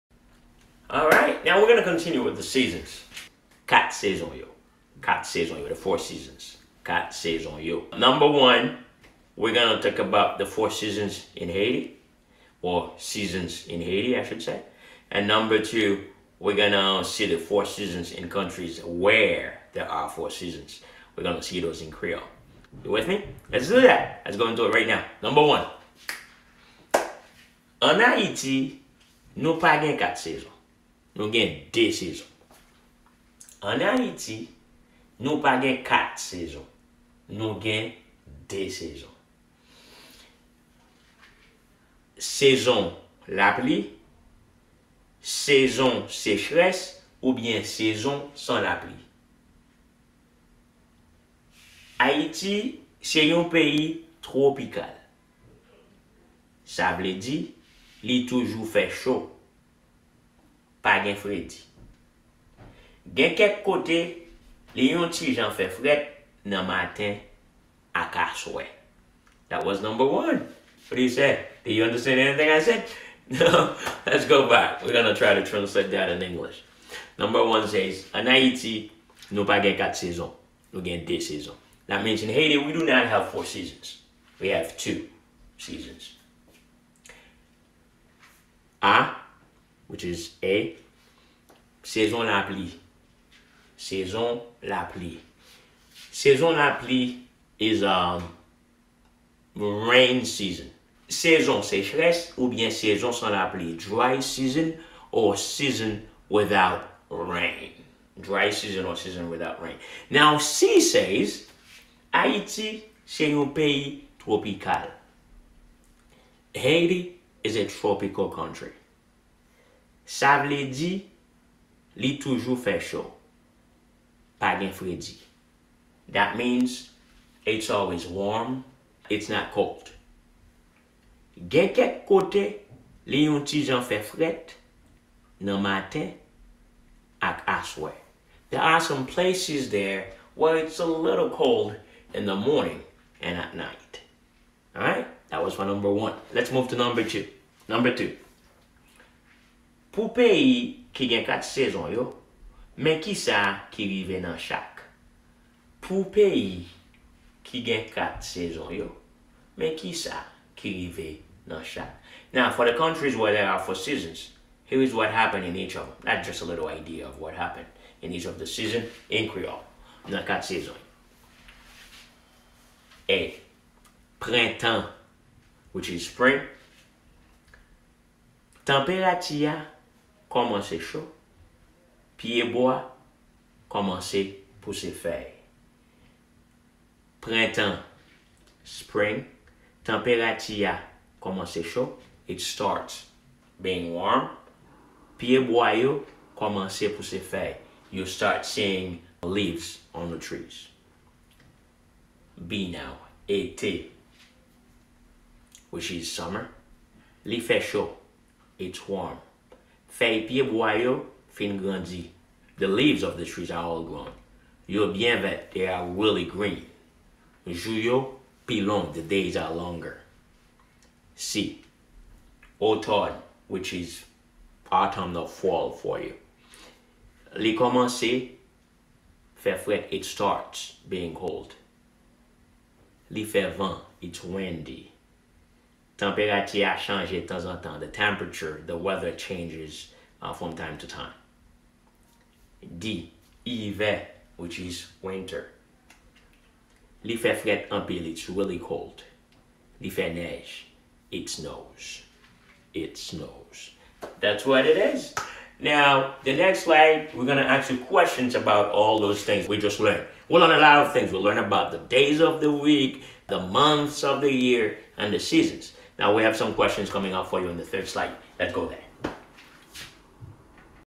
Kat Sezon Yo - LESSON 14B
Mr Emmanuel Darius teaches the fundamentals of Haitian Creole from English to Haitian Creole.The Udemy Course can be accessed